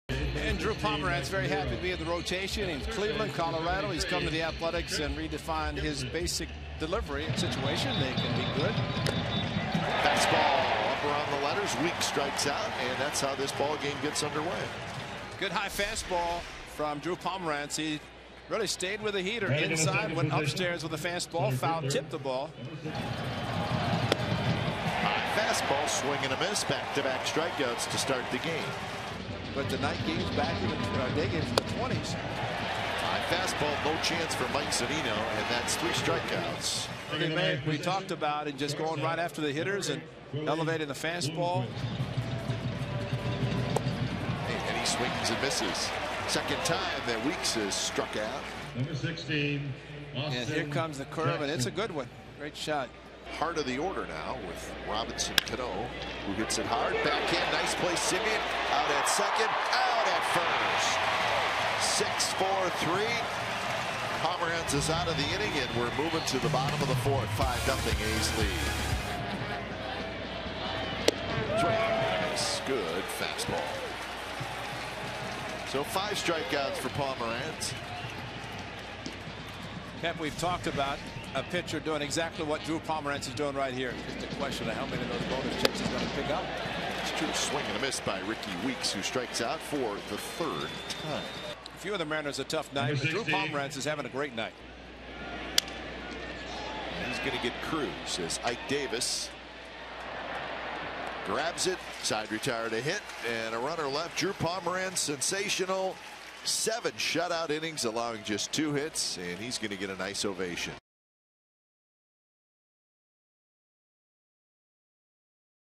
SEA@OAK: Pomeranz throws gem in first outing of 2015
4/10/15: Drew Pomeranz shuts down the Mariners over seven innings, giving up only two hits and striking out six